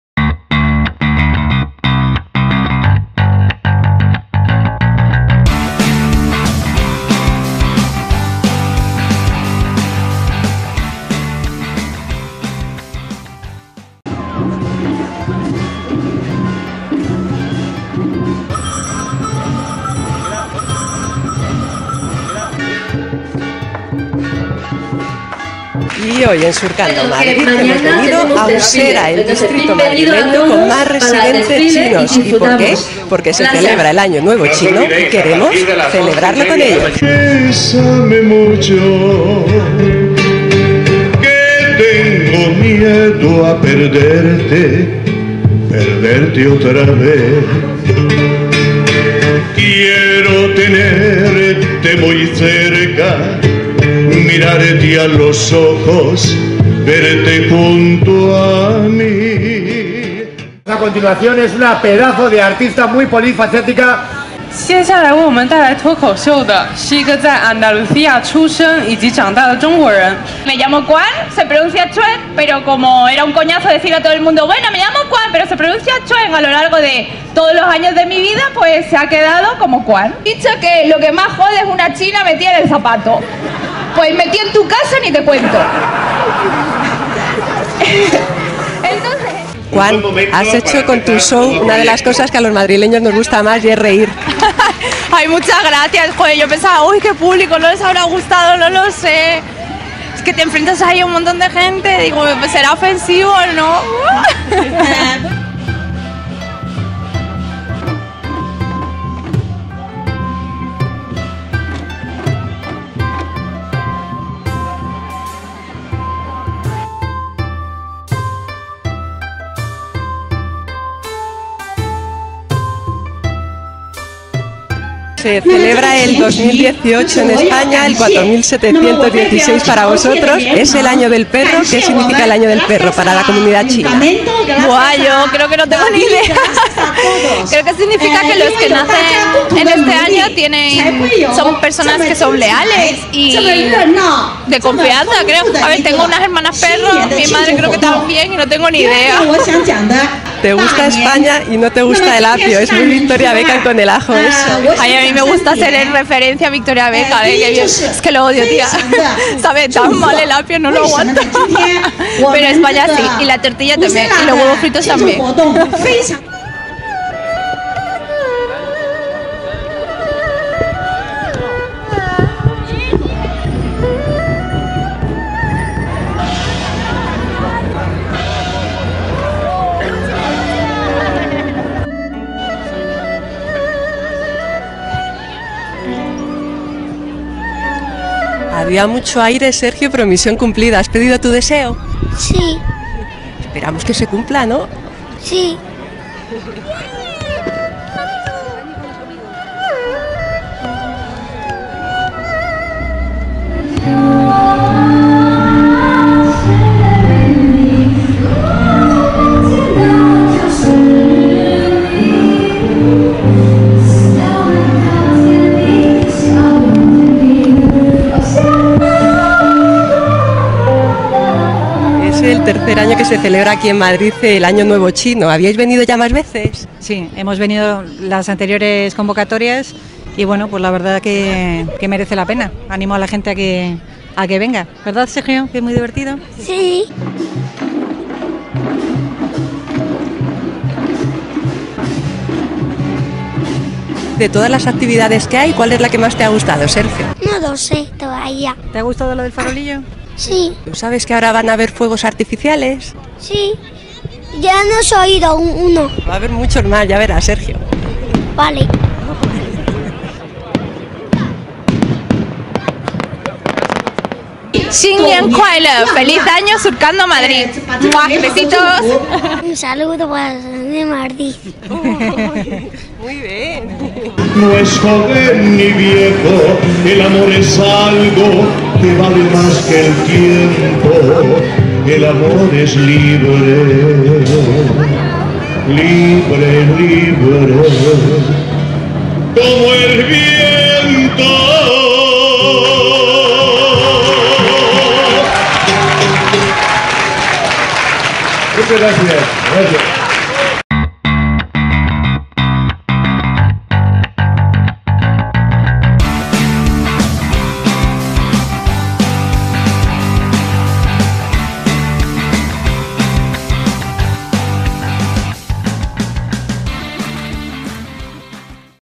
Celebrando el Año Nuevo Chino en Usera
Reportaje grabado en el madrileño barrio de Usera con motivo de la celebración del Año Nuevo Chino. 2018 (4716, según el calendario tradicional chino): el Año del Perro de tierra. Imágenes del Desfile de Año Nuevo, Pasacalles. Ruta Gastronómica y Cultural. Conciertos. Ceremonia de los farolillos flotantes, monólogo de Quan Zhou. Fuegos artificiales Lago de Pradolongo.

¿Quieres más?